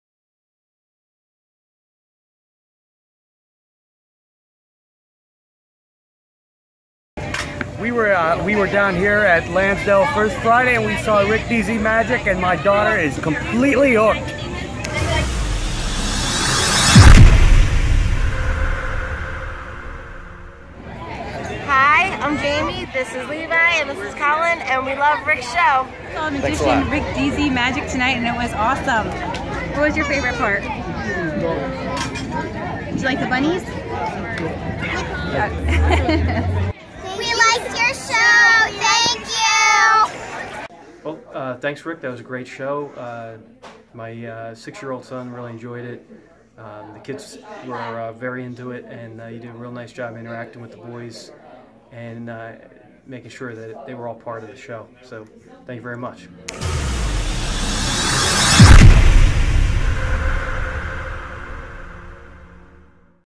Philadelphia Birthday Party Magician Rick DZ magic
Check out these glowing reviews for Rick DZ Magic, available for Birthday parties and other events in the Philadelphia area, including Montgomery, Lehigh and Bucks Counties